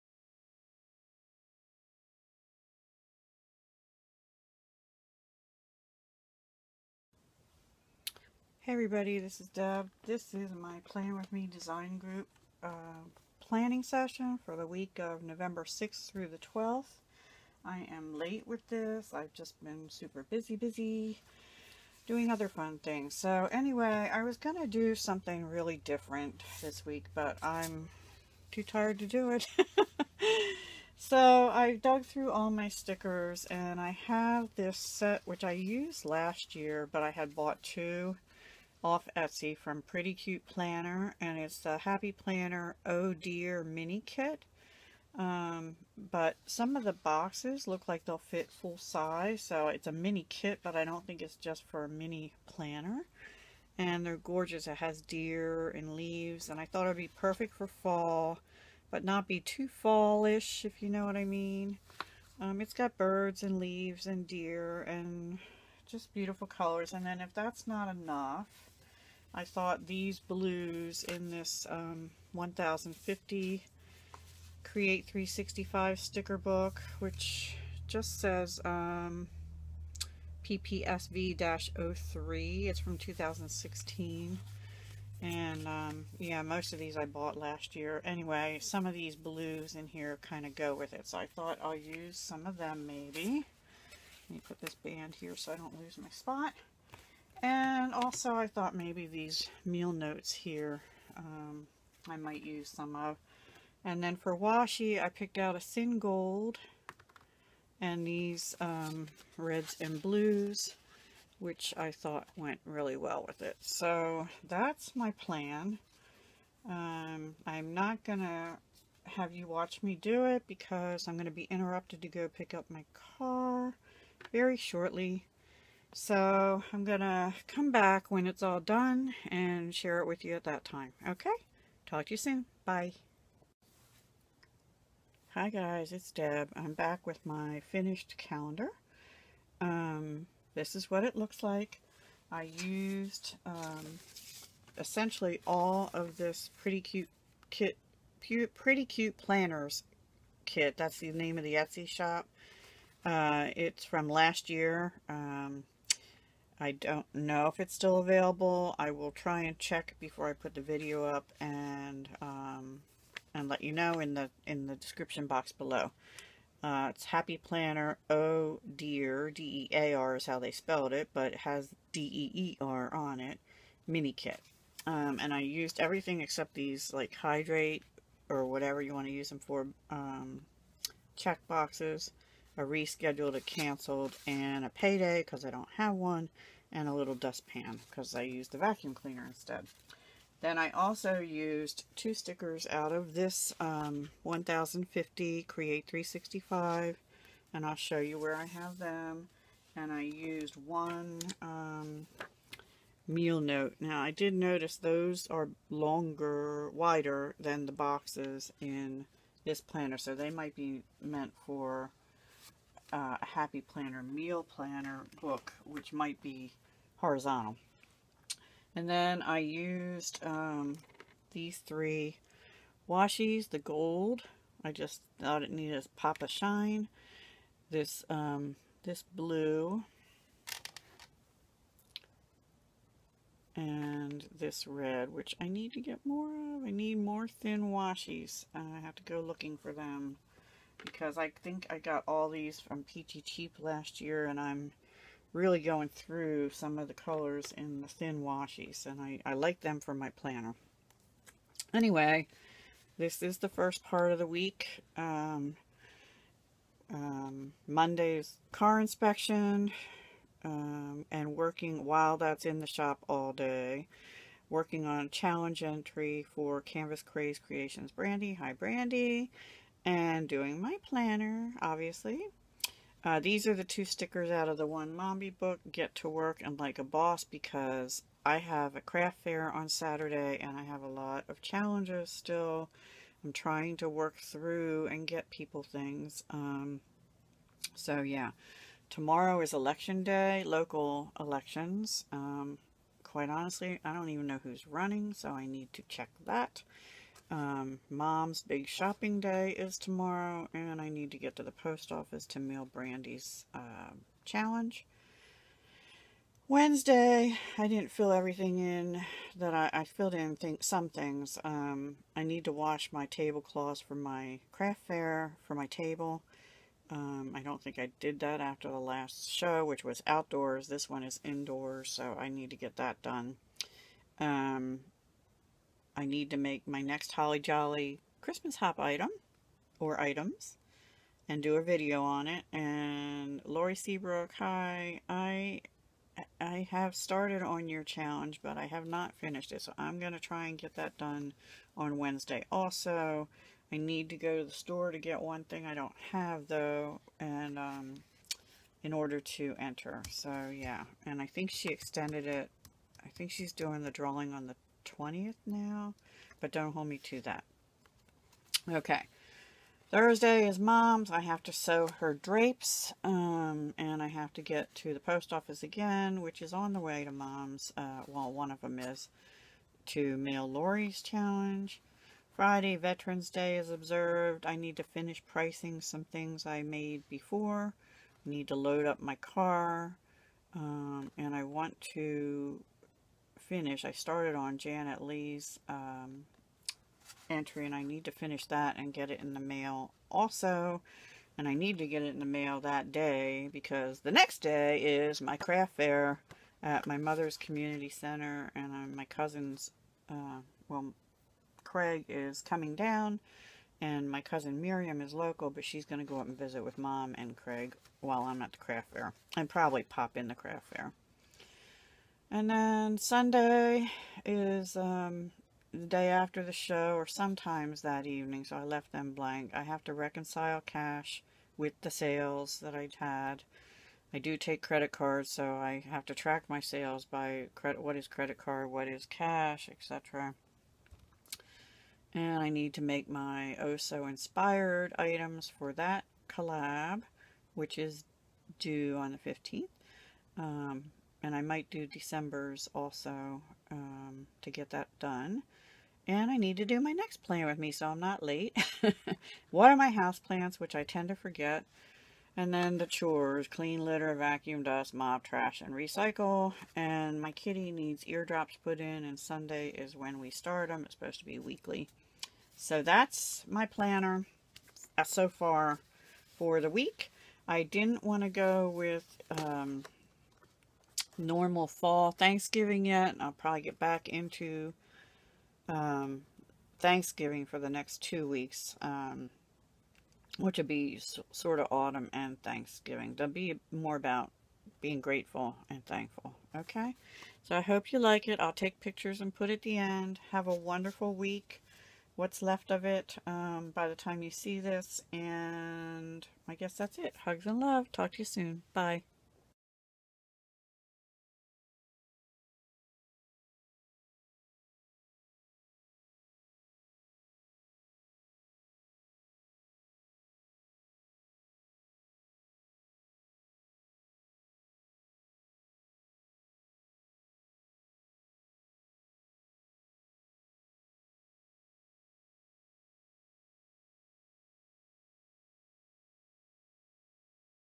V418 PWMDG Happy Planner Week of 11/6 thru 11/12/17
Hope you like my layout this week using stickers called Oh, Dear (Deer) from Pretty Cute Planner (Etsy shop).

Be sure to check all the other planner layouts by members of the group. Links are below:
23. Creative Designs by Jessica

Thanks so much for being part of my YouTube family! MyKalliTuxedoKatIsTheCutest